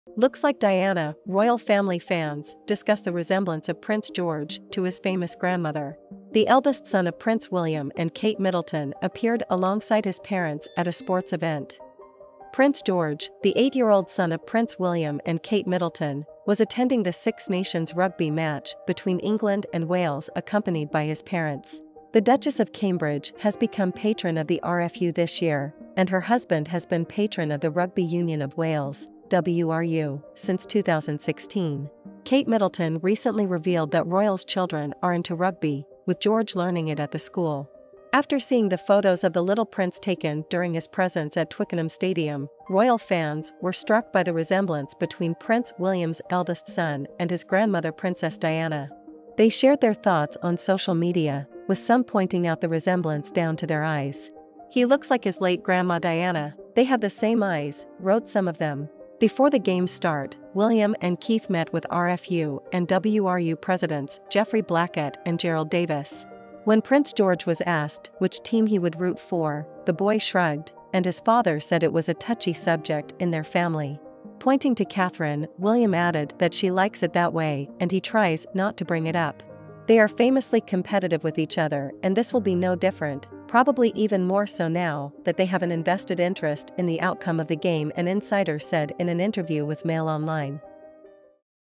“LOOKS like Diana!”: Fans discuss the resemblance of Prince George to his famous grandmother
Kate Middleton recently revealed that royals’ children are into rugby, with George learning it at the school. After seeing the photos of the little prince taken during his presence at Twickenham Stadium, royal fans were struck by the resemblance between Prince William's eldest son and his grandmother Princess Diana.